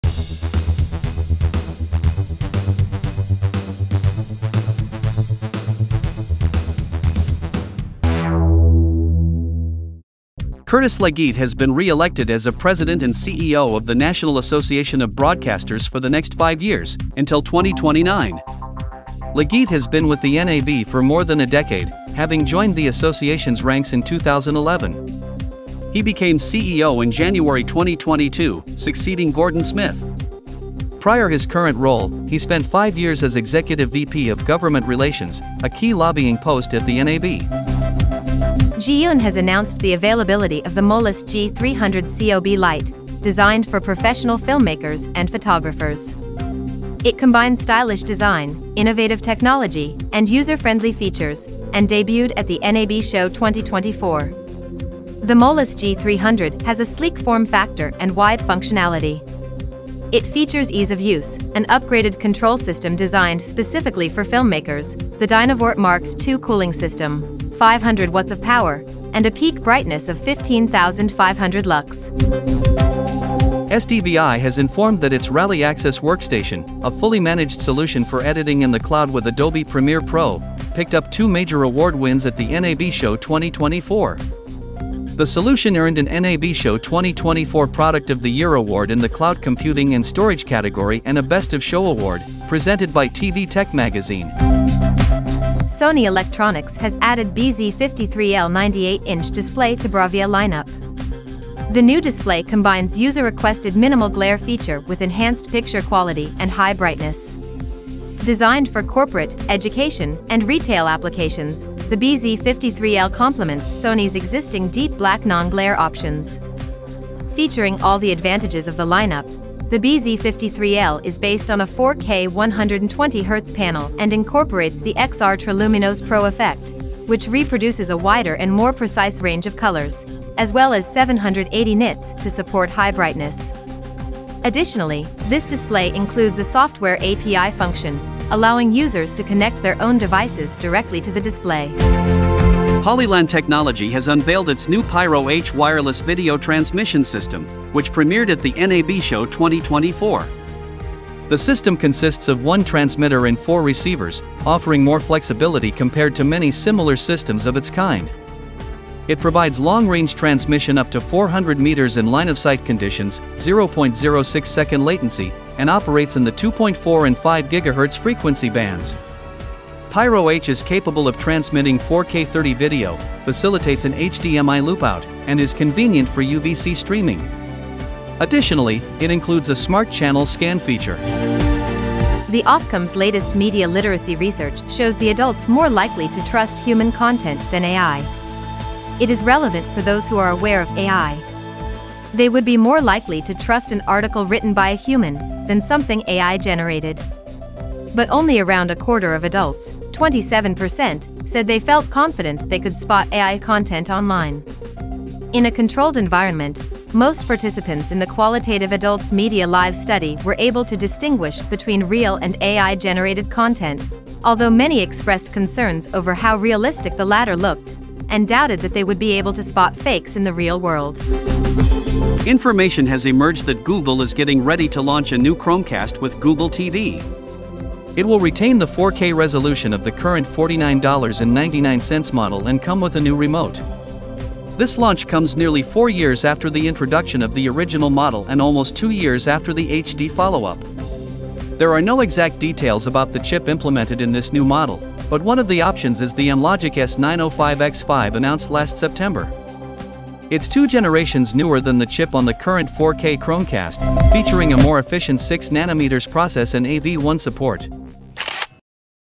Briefly 85 by MediaVision News
Hello everybody! After the NAB Show 2024, 'Briefly' returns to its normal schedule. Watch our brief news on Monday, Tuesday, Thursday, and Friday every week. In this particular 'Briefly', you will find news from the NAB, Zhiyun, SDVI, Sony, Hollyland, Ofcom, and Google.